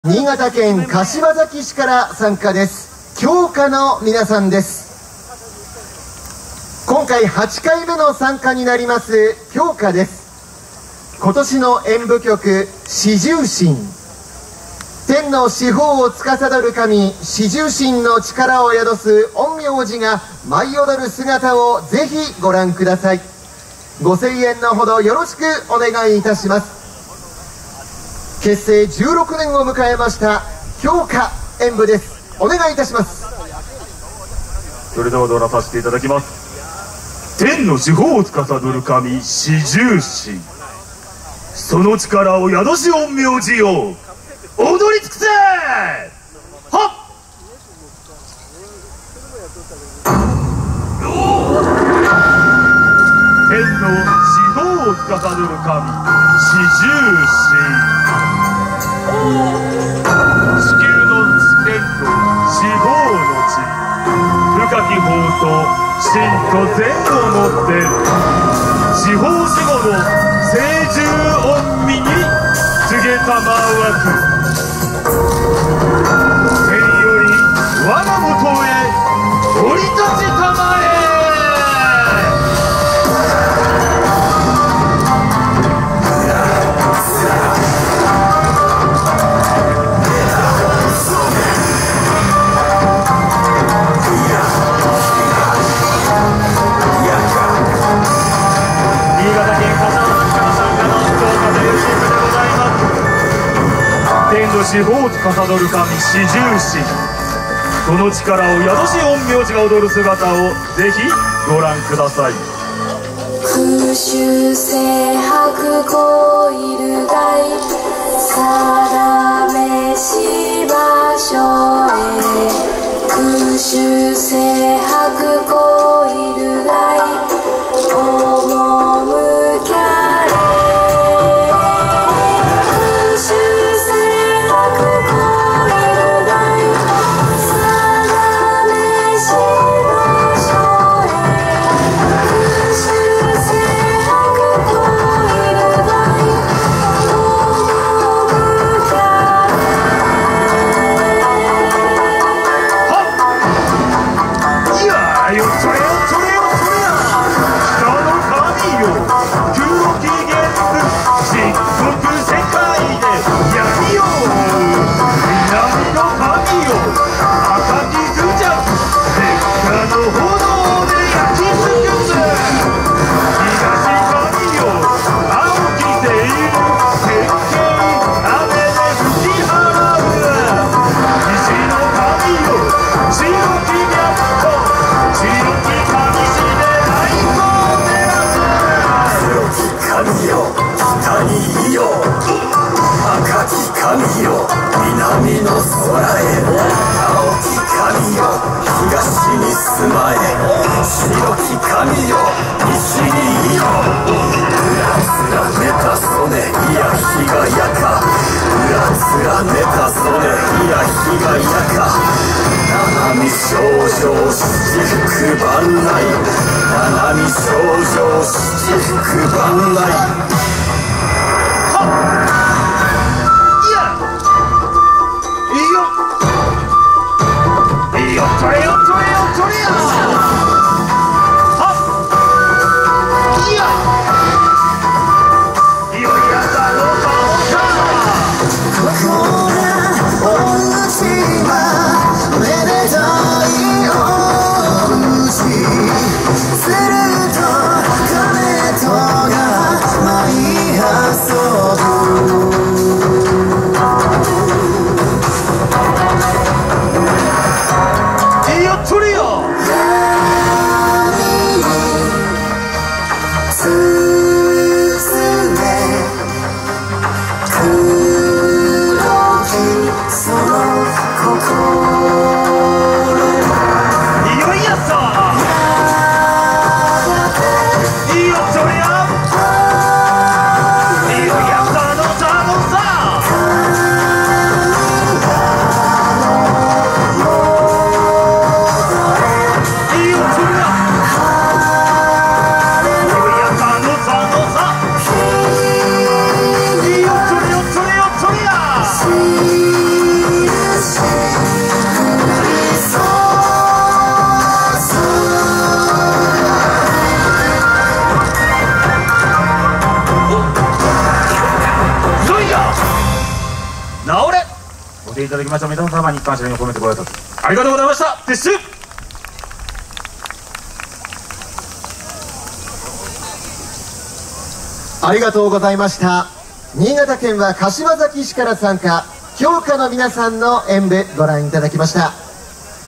鏡華 @ よさこいとやま (城趾公園) 2019-08-04T16:09
2019年8月4日(日)、富山県富山市で開催された「よさこいとやま」(城趾公園)での「鏡華」さんの演舞です。